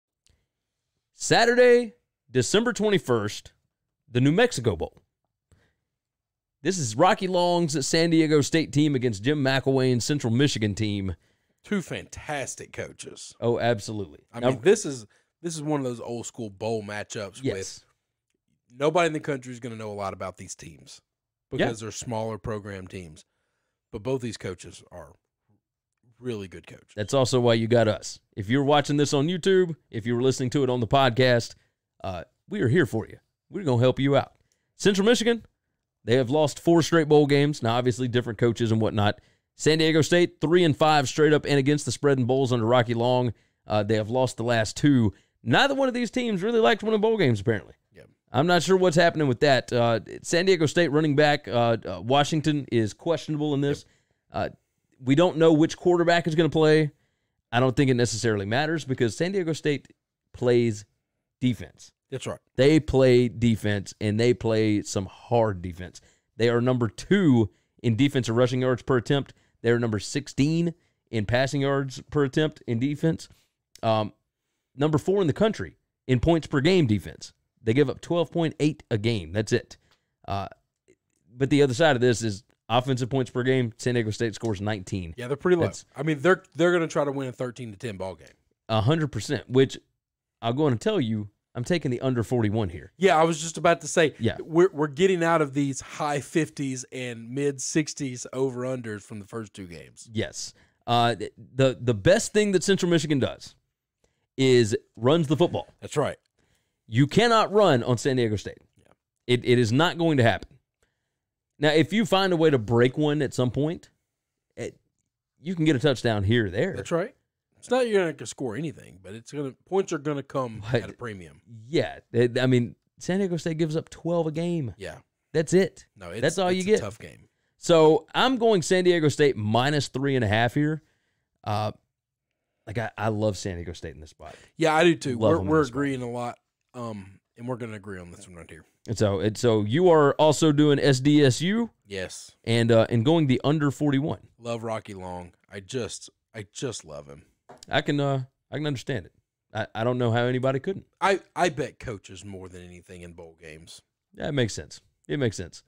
WCE: 2019 New Mexico Bowl (San Diego St vs Central Michigan) Preview & Against the Spread Picks
Gary and Chris preview College Football's 2019 New Mexico Bowl between Central Michigan and San Diego St, taking place on Saturday, December 21st, with picks against the spread, point totals, and a look into different stats for each team, and more!